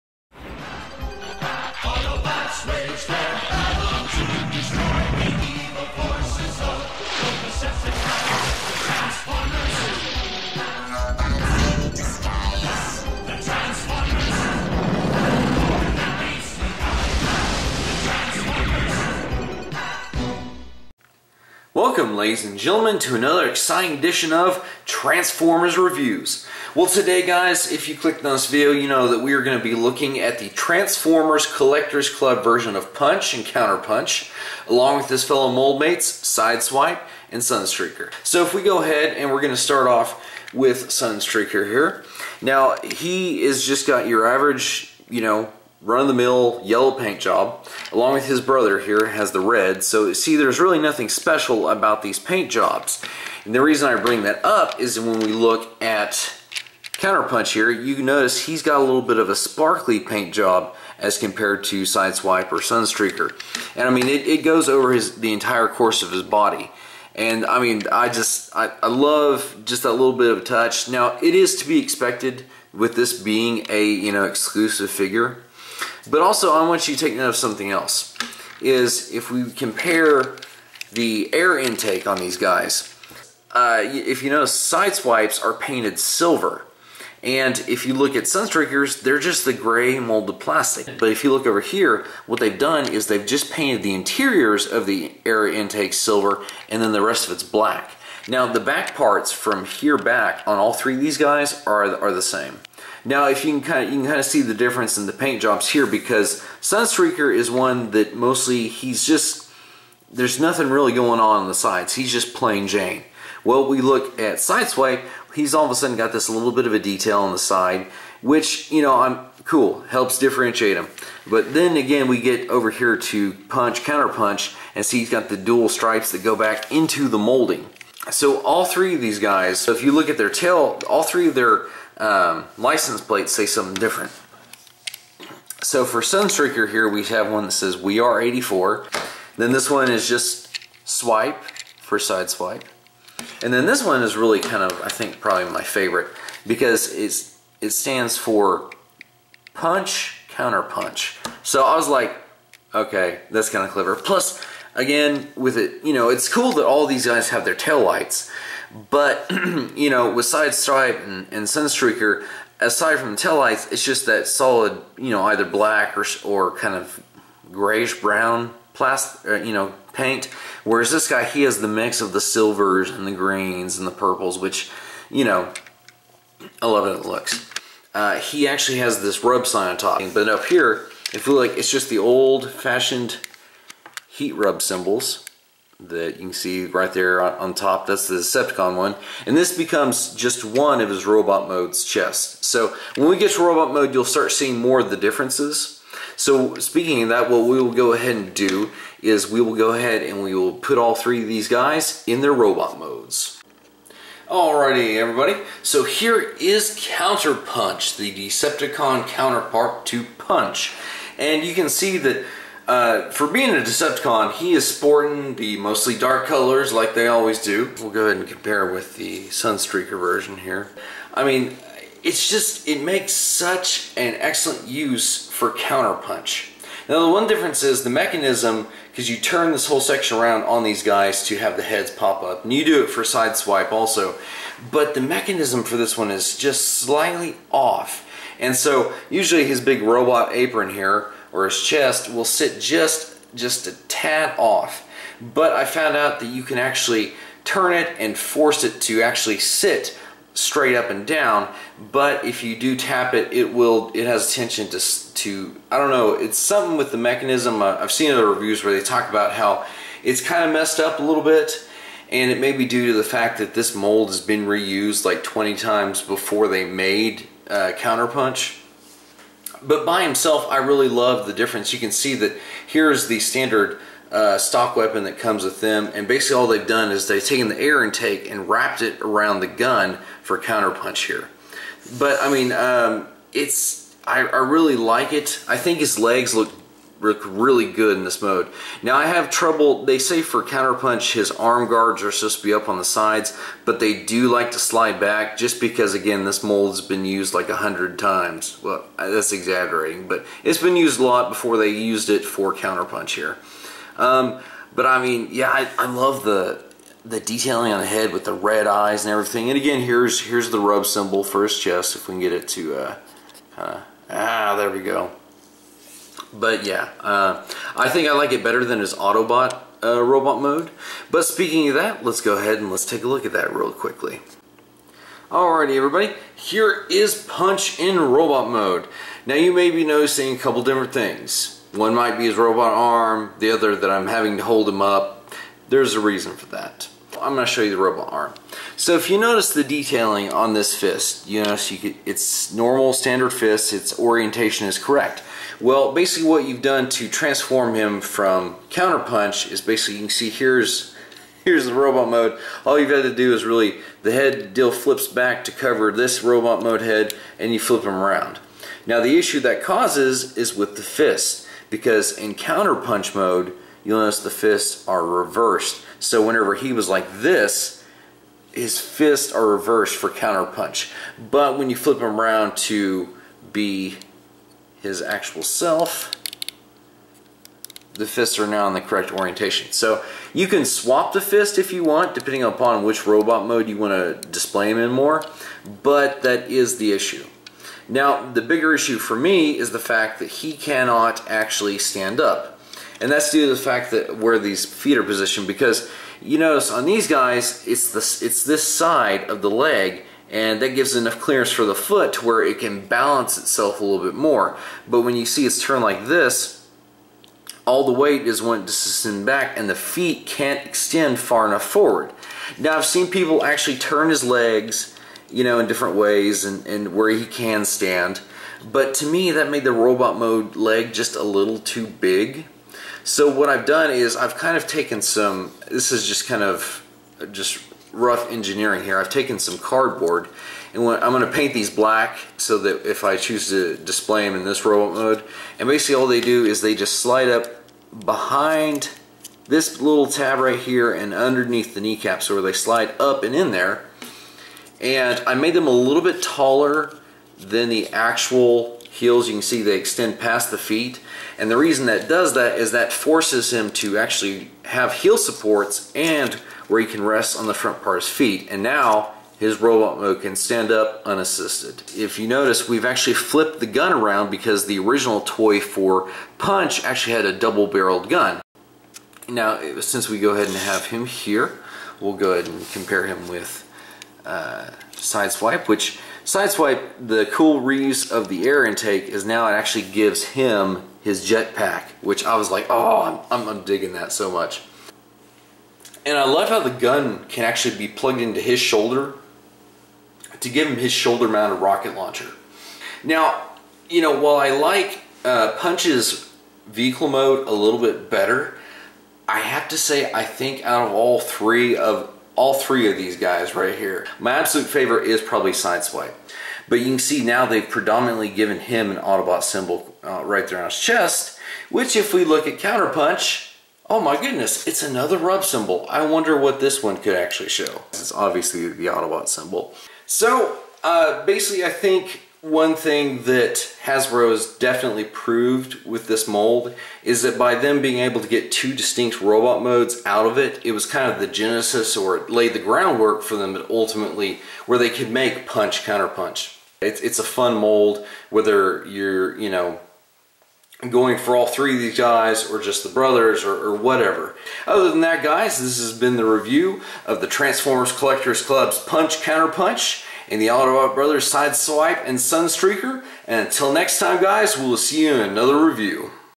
TFCC Puch Counter Punch with Sideswipe and Sunstreaker
So here is the highly sought after Transformers Collector's Club Punch/Counter-Punch. Alongside him today is the classic Autobot Brothers Sideswipe and Sunstreaker. We are going to take a look at the differences between the three to see how a single mold can make three or four different robots with just a few tweaks. :)